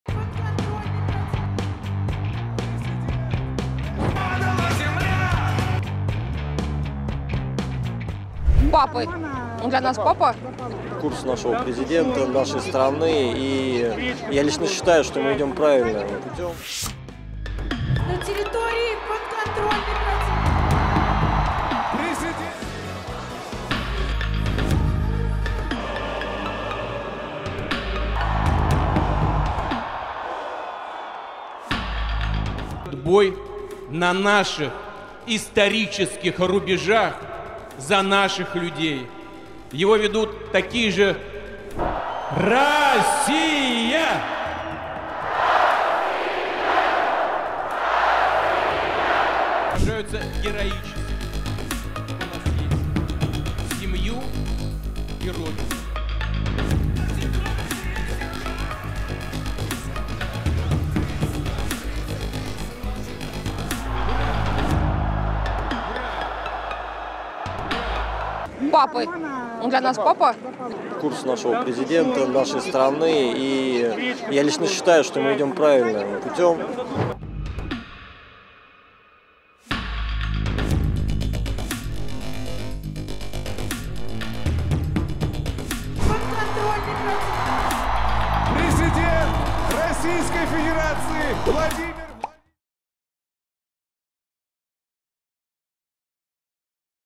영하 15도에도 수만 명 참석…애국콘서트 진짜 스타는 '푸틴' / 연합뉴스 (Yonhapnews)
영하 15도에도 수만 명 참석…애국콘서트 진짜 스타는 '푸틴'

    (서울=연합뉴스) 러시아의 우크라이나 침공 1주년을 이틀 앞둔 22일(현지시간), 모스크바 최대 축구경기장에서 애국심을 고취하는 콘서트가 열렸습니다.

    영하 15도 혹한 속에도 수만 명이 모였는데요, 콘서트의 주인공은 단연 블라디미르 푸틴 러시아 대통령이었습니다.

    푸틴은 관람객의 연호 속에 마치 연예인처럼 무대 중앙에 등장했습니다.

    그는 환호하는 관람객에게 "우리의 역사적 영토, 우리의 인민을 위한 전투가 현재 진행되고 있다"며 군을 지지하는 모든 이들이 '조국의 수호자들'에 해당한다고 강조했습니다.

    행사에 참가한 마리야라는 이름의 여성은 외신 인터뷰에서 "푸틴은 저에게 아버지와 같다"고 말했고, 다른 남성은 "그(푸틴)가 올바른 방향으로 가고 있다"고 했습니다.

    '조국 수호자들에게 영광을'이라는 이름의 이 행사는 러시아 공휴일인 '조국 수호자의 날'(2월 23일) 하루 전에 열렸습니다.

    미국 정치전문매체 폴리티코는 "비슷하게 보이는 버스들이 줄을 지어서 비슷하게 생긴 사람들을 내려주고, 이들이 비슷한 크기의 러시아 국기를 흔드는 전통적 풍경이 우드스톡(미국 록 음악 축제)보다는 북한에 가깝게 보인다"고 꼬집었습니다.

    [제작 : 진혜숙·이금주]
    [영상 : 로이터·폴리티코 홈페이지]